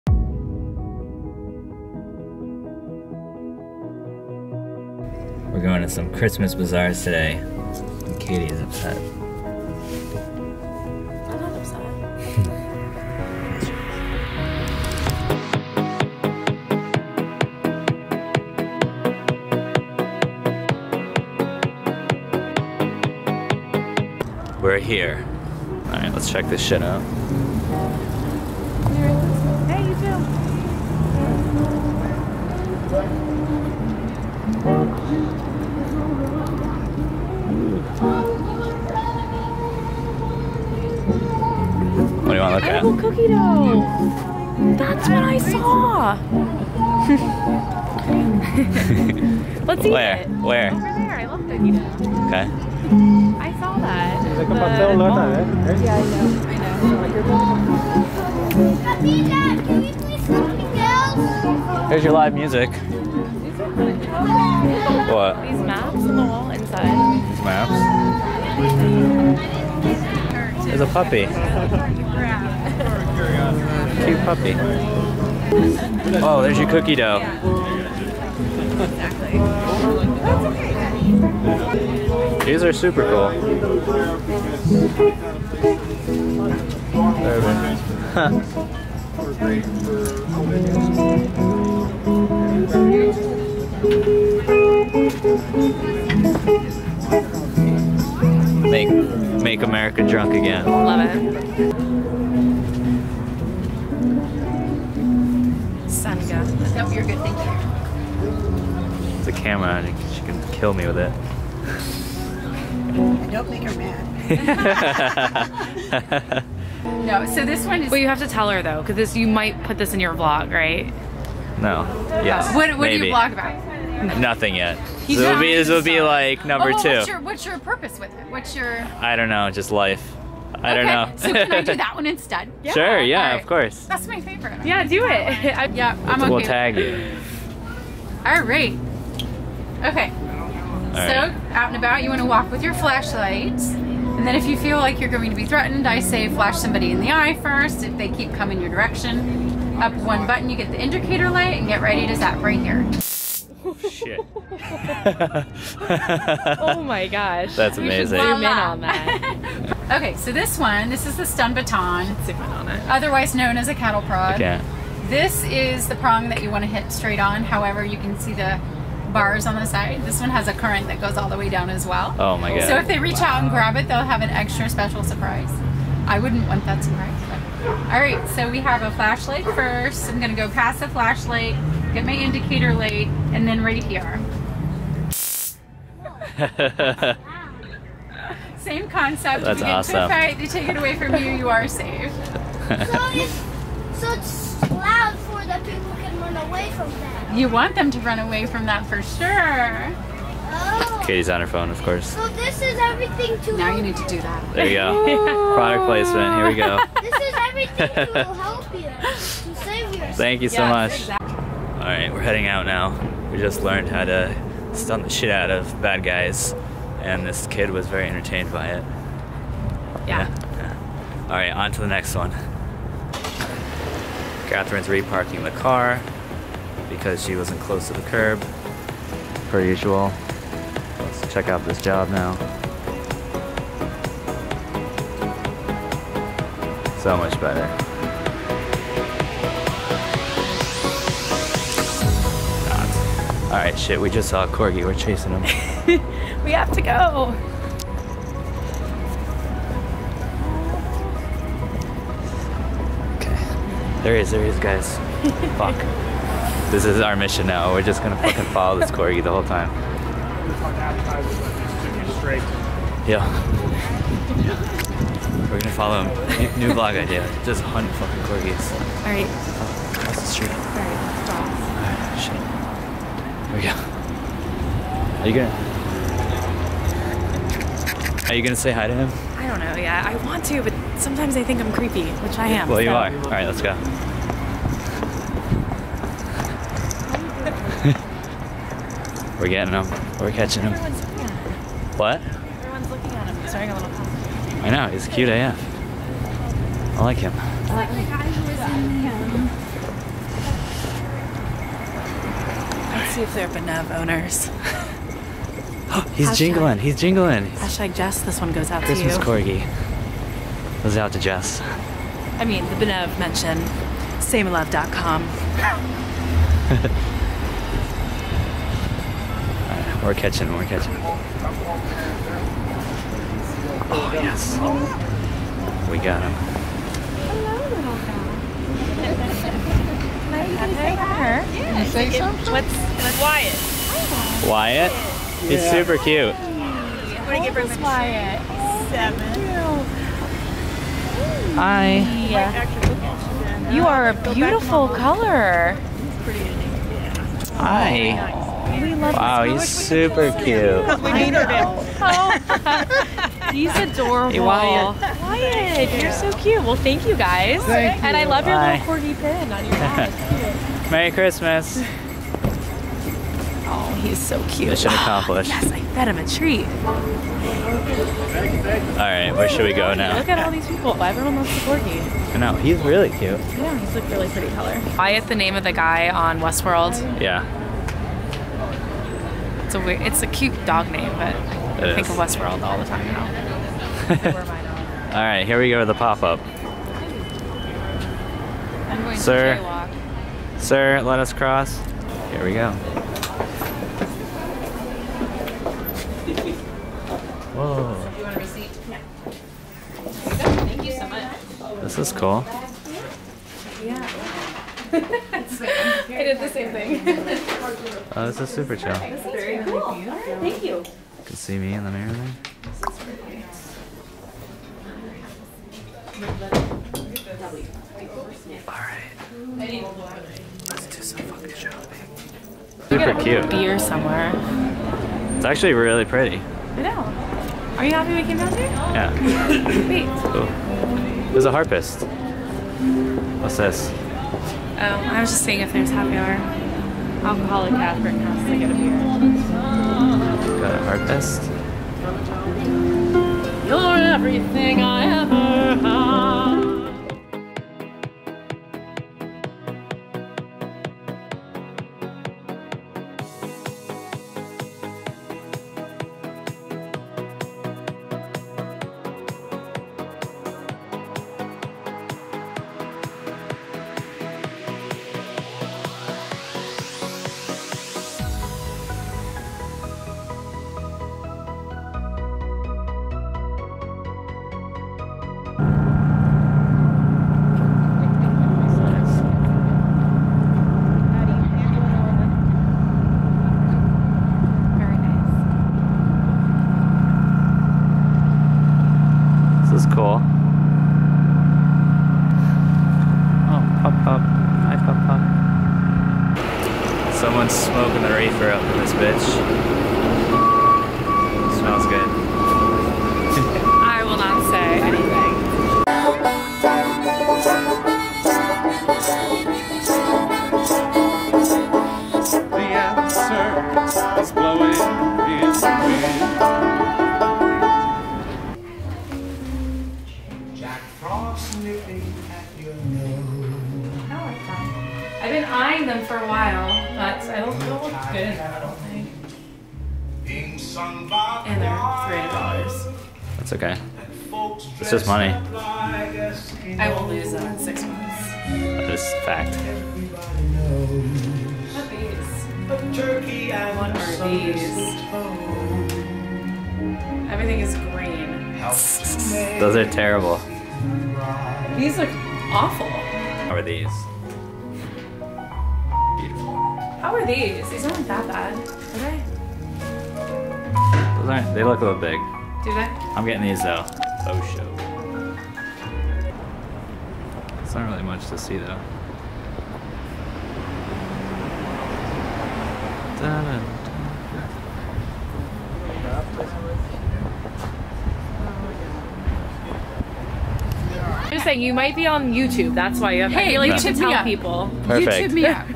Chasing Corgis in Old Man's Town
Went to a couple Christmas Bazaars in Old Town Alexandria, VA and saw a Corgi!

Shoutout to Valerie for giving us a demonstration on how to taze bad guys!

Gear used in this video:
My Camera
My Lens